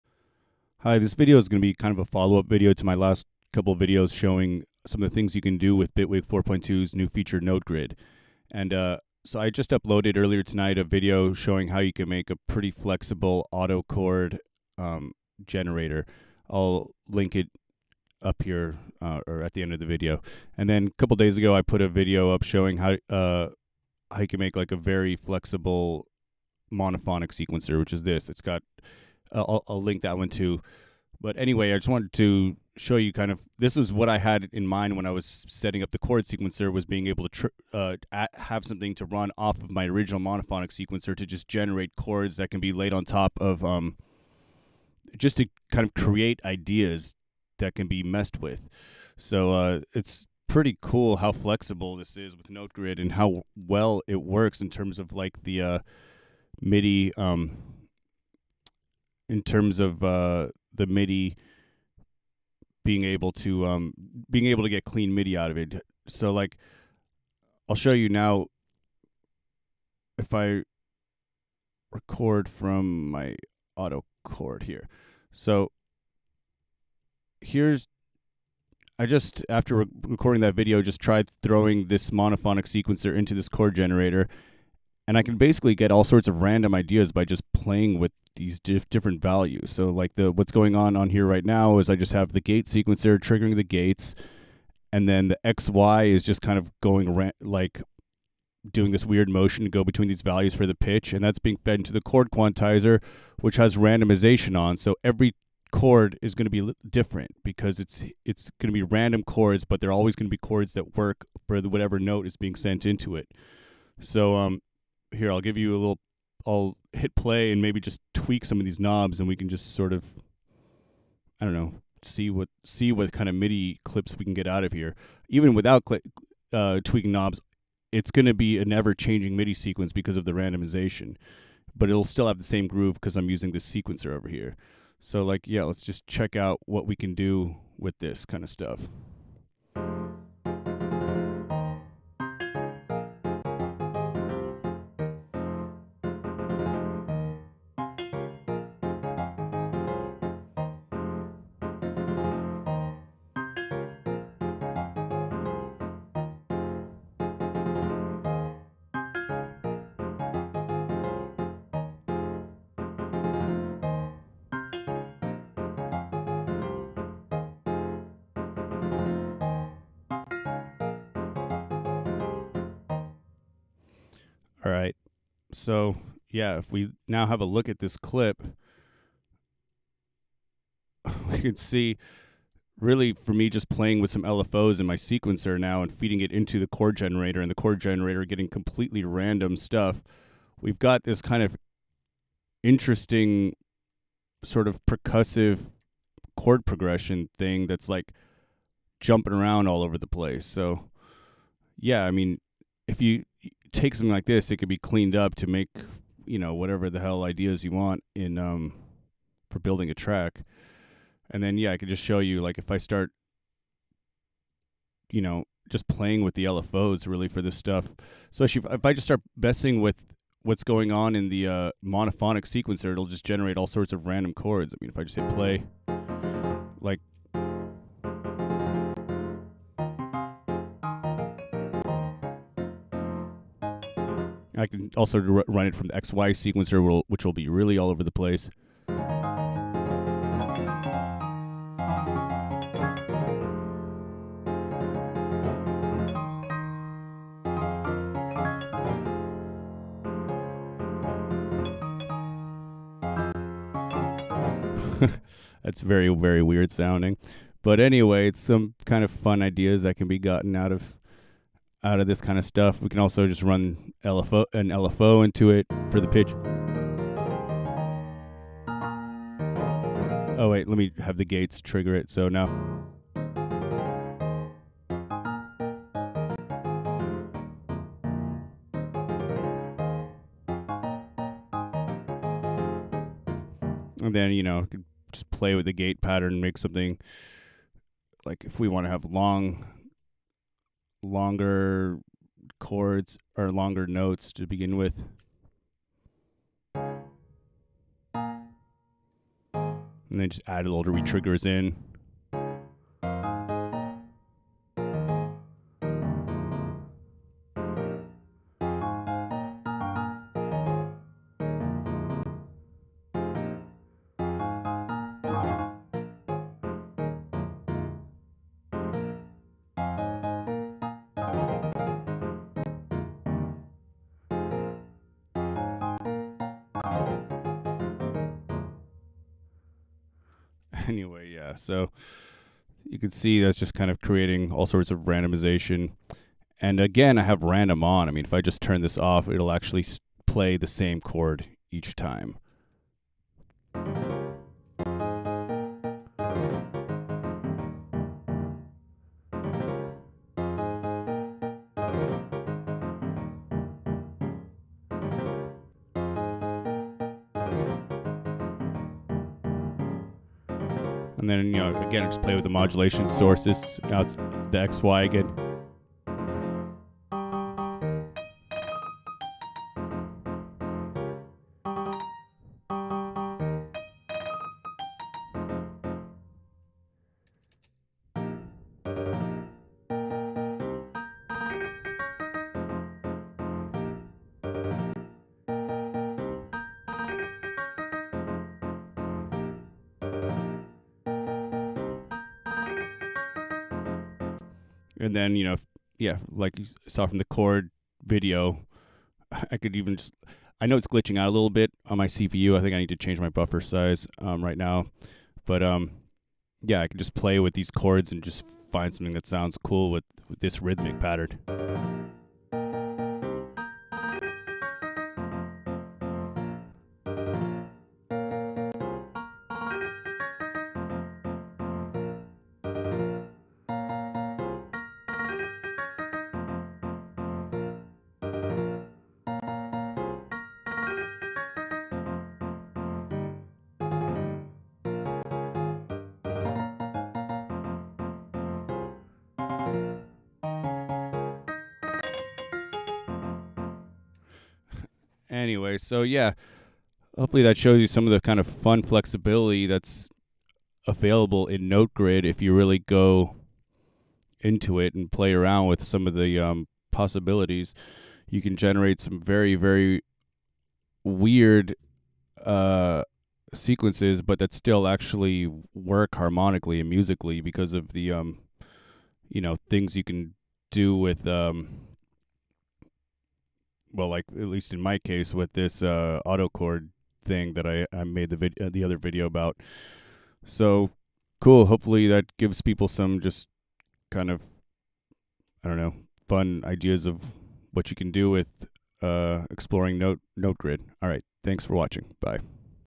Bitwig's Note Grid - Demonstrating possibilities for creating glitchy/ wacky chord progresions.
This video is just looking at some of the things that are possible with the new Note Grid device in Bitwig.  In this video, I combine my monophonic sequencer into my chord cheat device to create very wacky chord progressions.